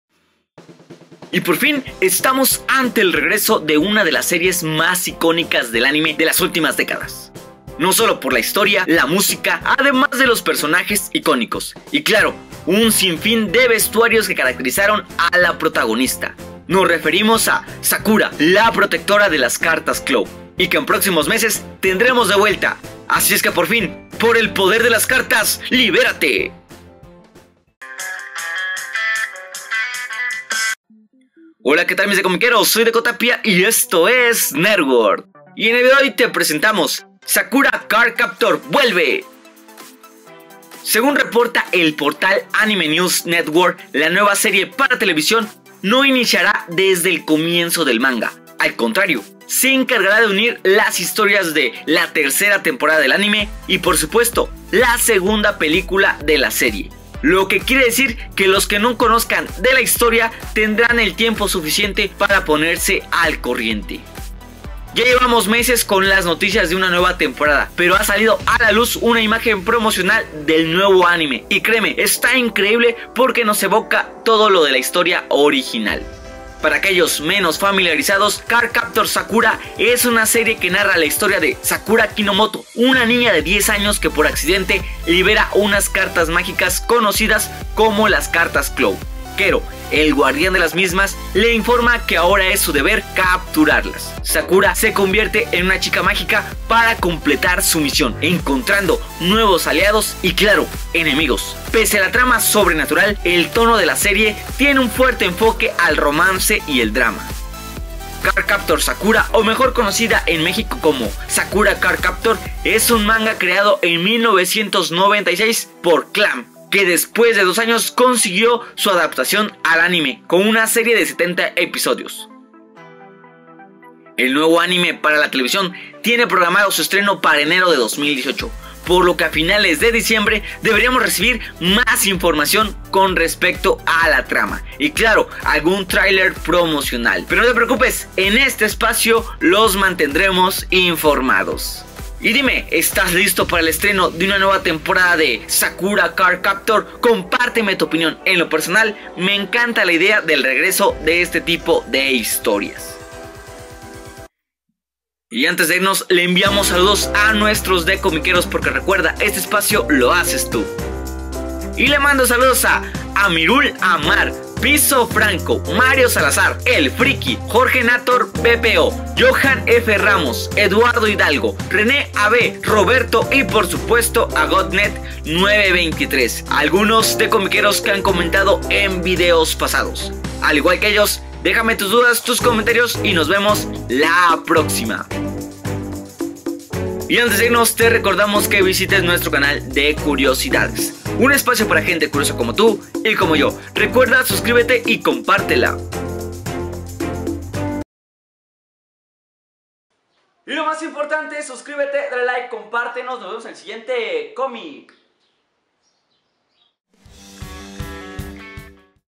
Sakura CardCaptor | VUELVE | Nuevo Anime ✨
En el vídeo de hoy hablaremos del regreso de SakuraCardCaptor con un nuevo anime que llegara a nosotros en 2018, tenemos nuevo poster el cual nos presenta que tendremos la esencia del anime original

Y nos vemos en el siguiente cómic!!

APOYANOS EN ESTE NUEVO PROYECTO

Déjanos tus comentarios, dudas o sugerencias y con gusto te saludamos hagamos crecer nuestro mundo comiquero